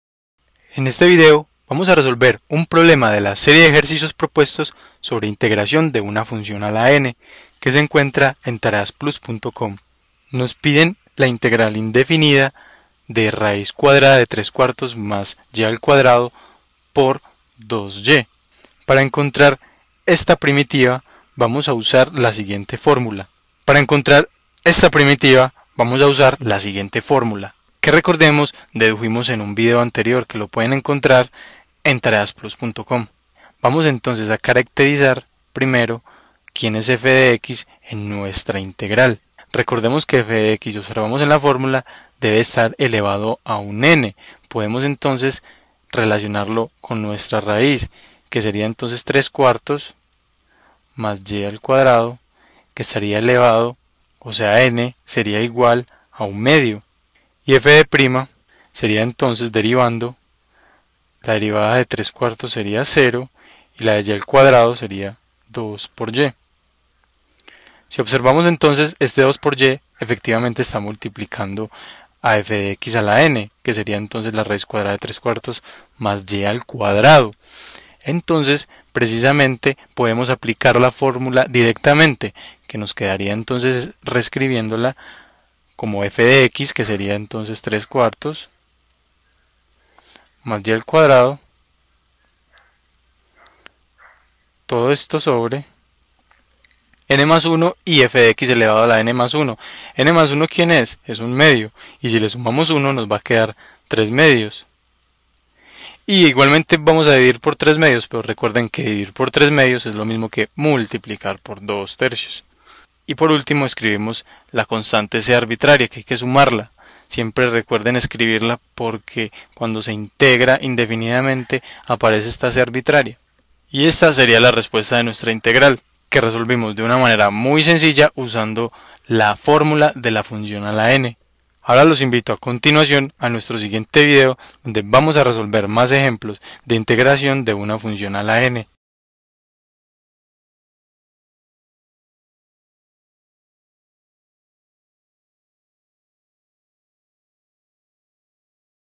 Integral de una función a la n. Ejercicio 3 de 15
Serie de problemas de cálculo integral sobre integracion de funciones a la n. Problema número 3 de quince ejercicios propuestos